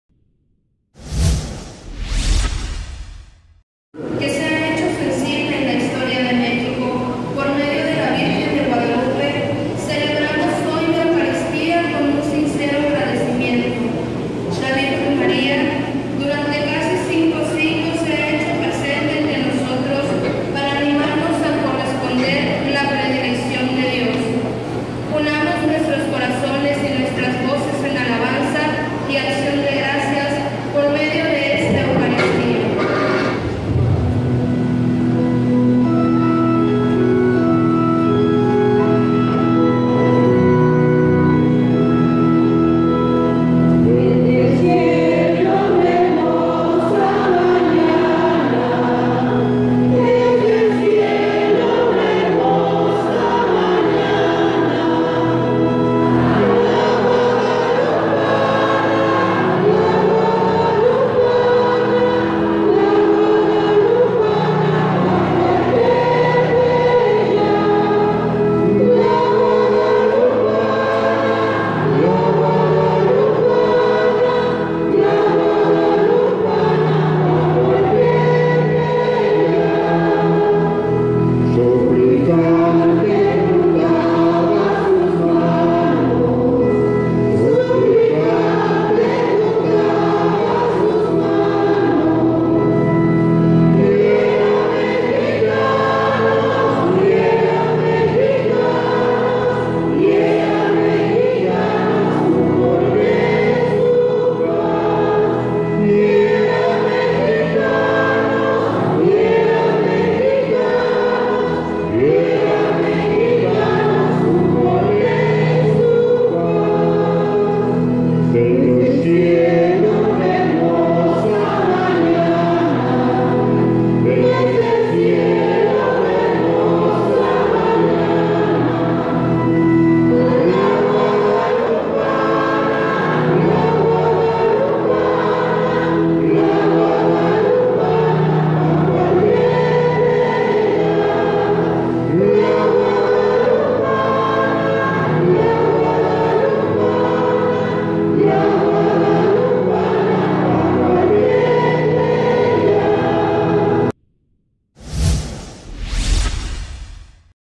Católicos de Coatzacoalcos celebran a la Virgen de Guadalupe
Coatzacoalcos, Ver., 12 de diciembre de 2016.- Desde las primeras horas de este lunes, decenas de católicos se congregaron en el Santuario de la Virgen de Guadalupe en Coatzacoalcos para celebrar su día.